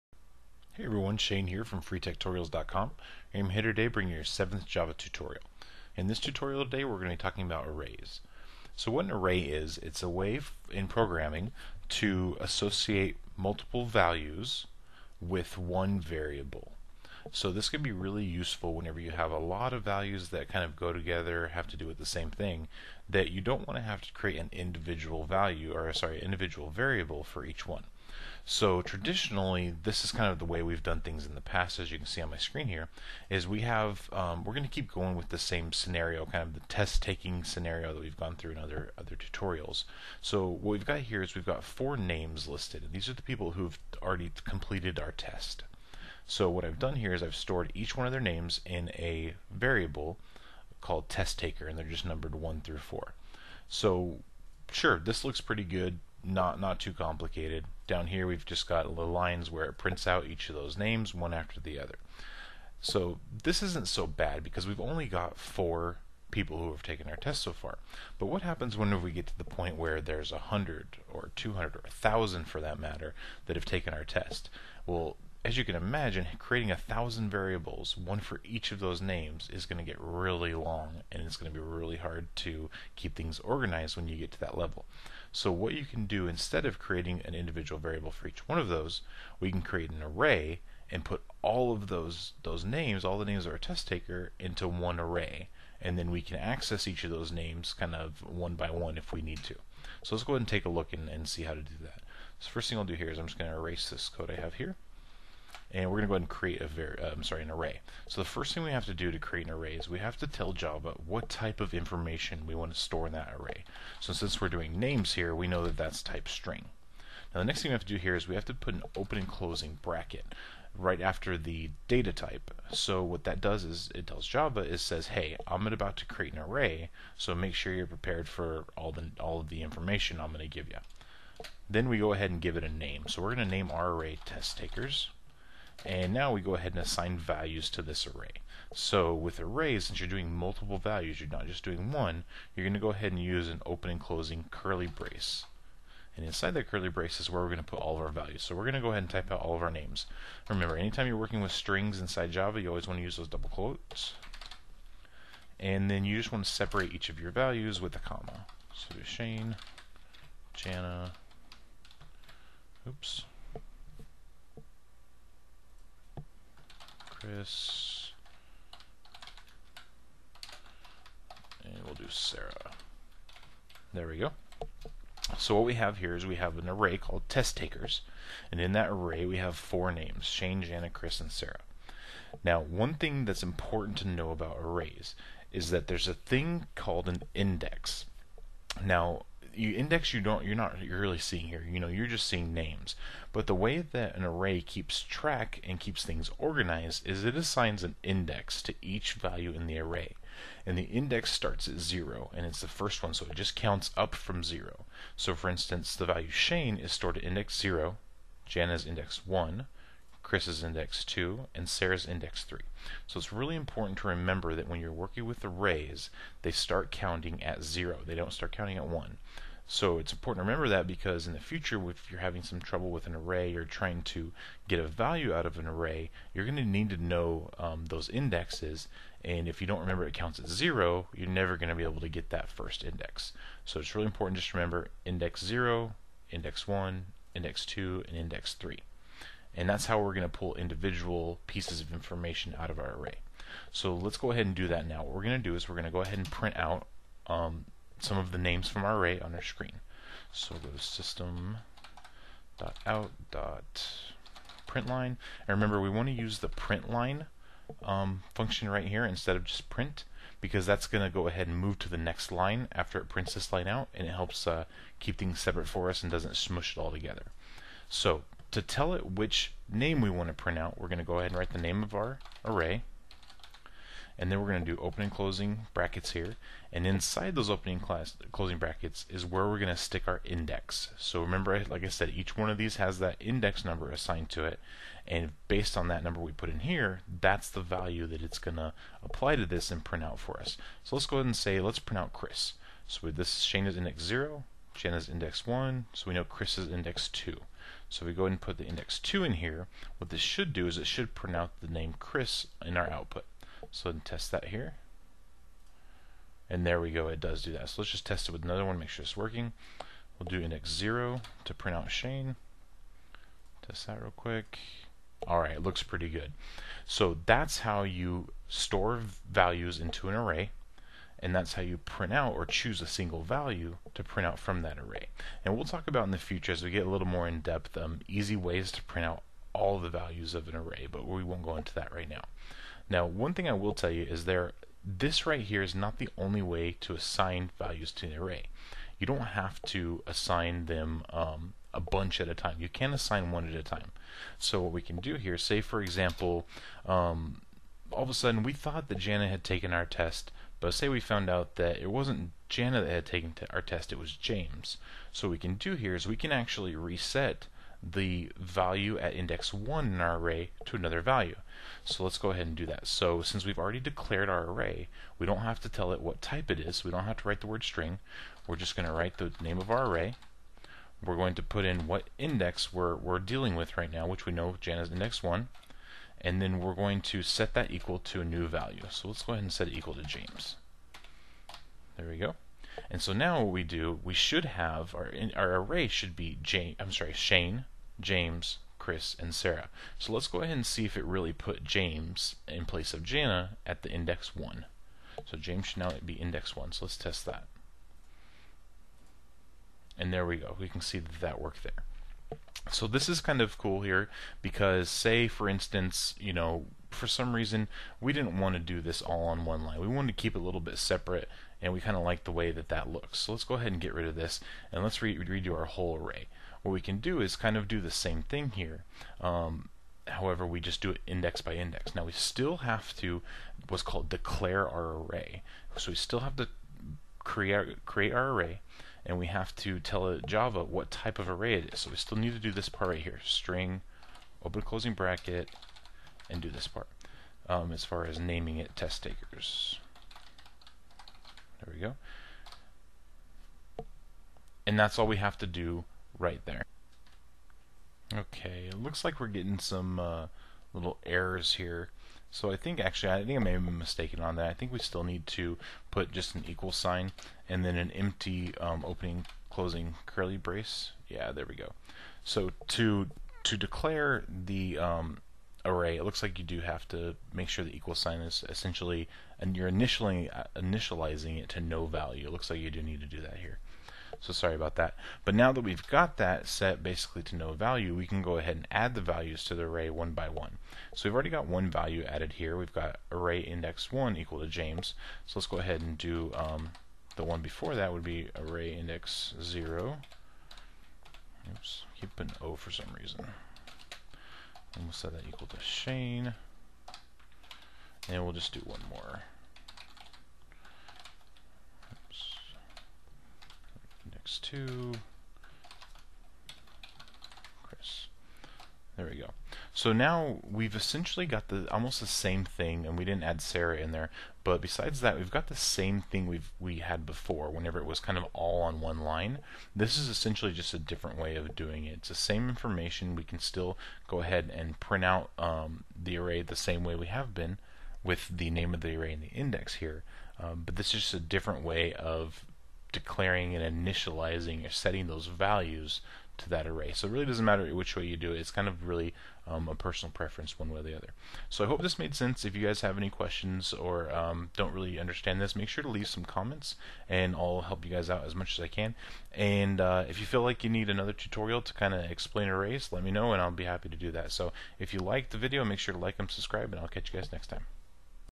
Beginner Java Tutorial #7: Arrays in Java
In this video we show how to declare and initialize arrays.